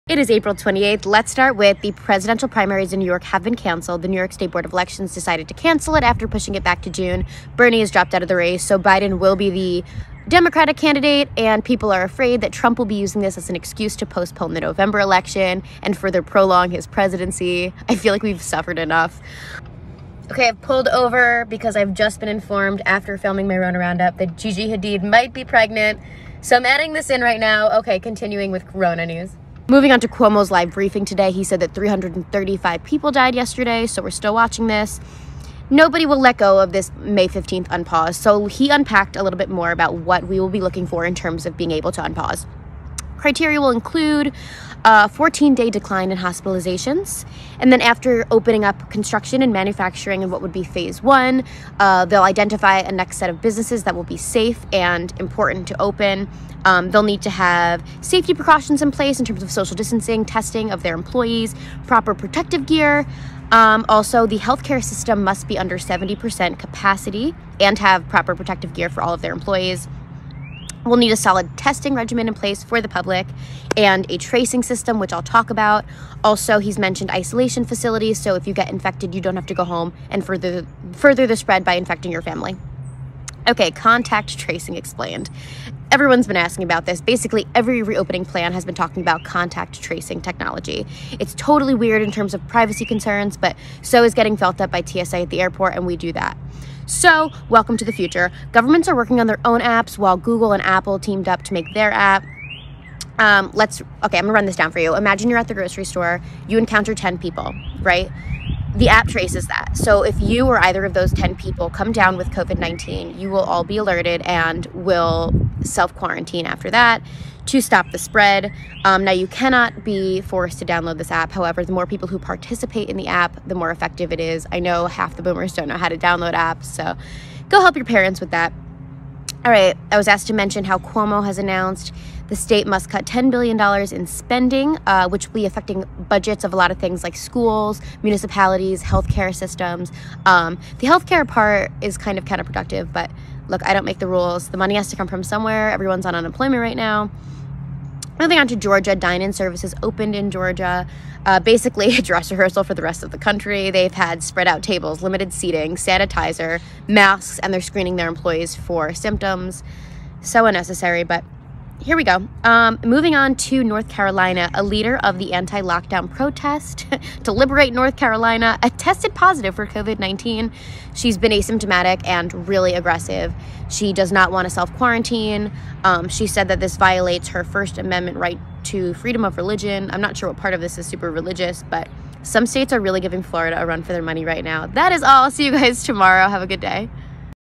RONA ROUNDUP 4/28
News brief in under 3 minutes—
From Trump to Cuomo to Gigi Hadid- I have it all covered.